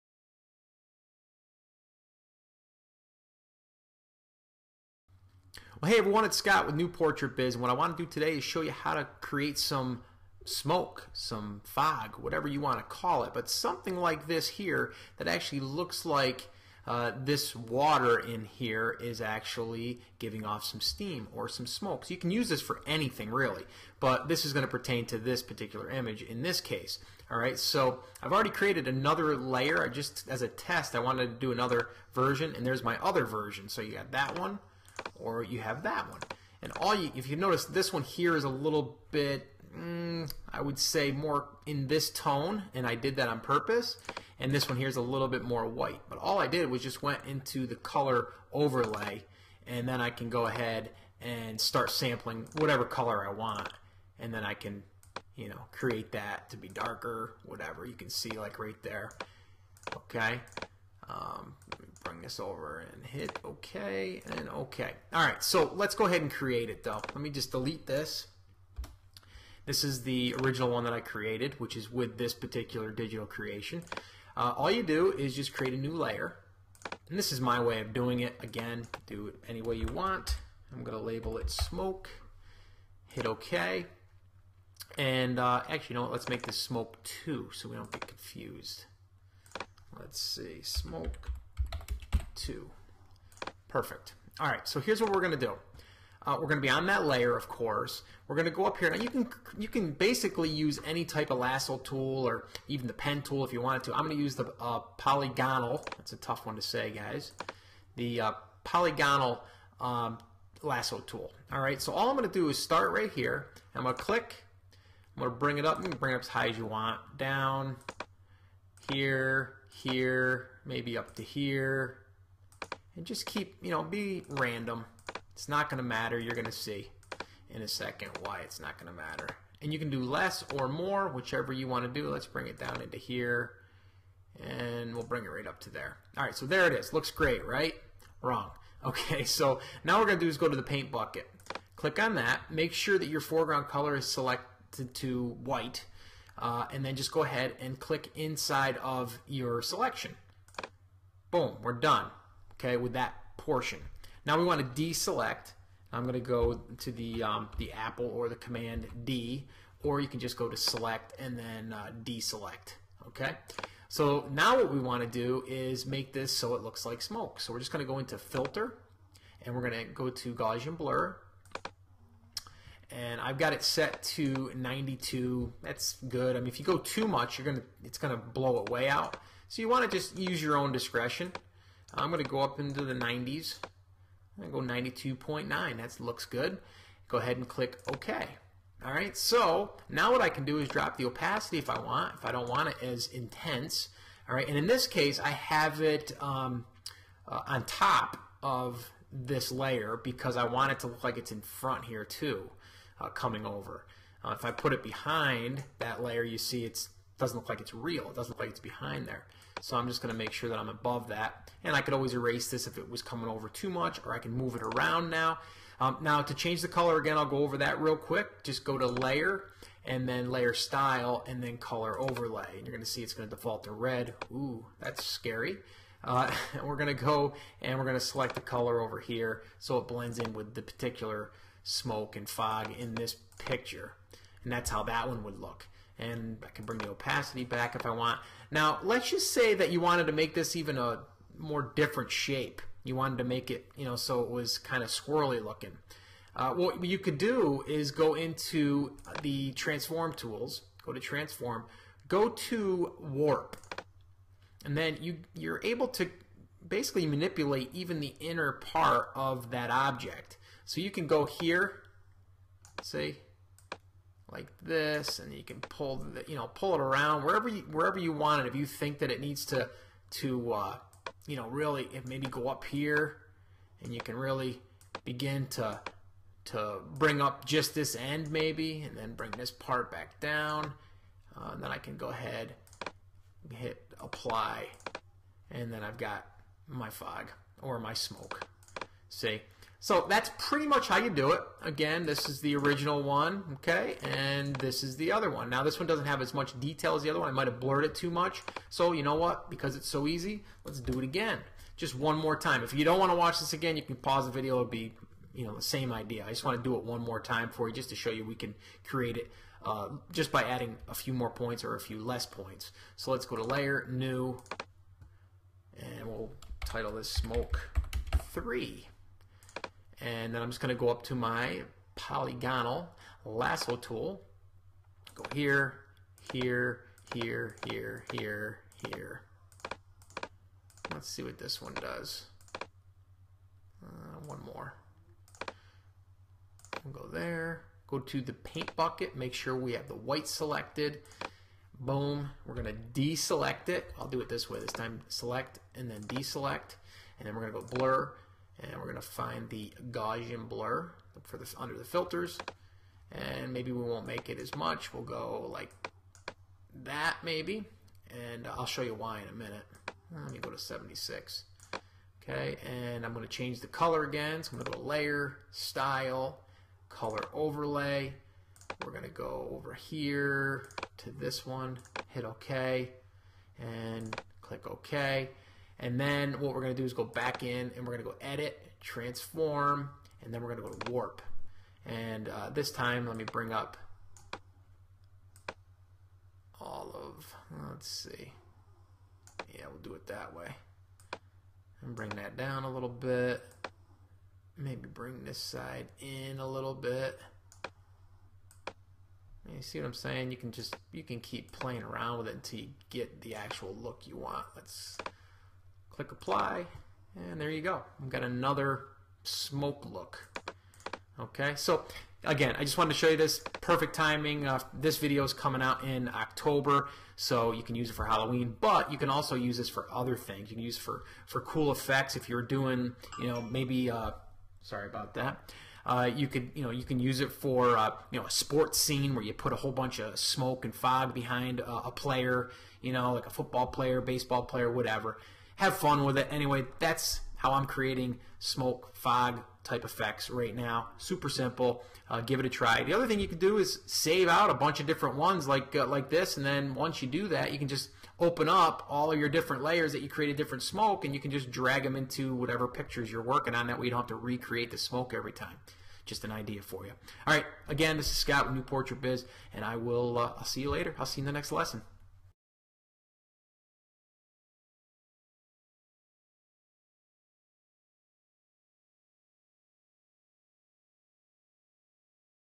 Simple Way To Create Smoke Effect In Photoshop
- I created this video to show how simple it can be to create smoke for any picture in Photoshop. There's other ways, but this is the easiest way I know how. You can use this for Halloween, sports, seniors and even modeling shoots.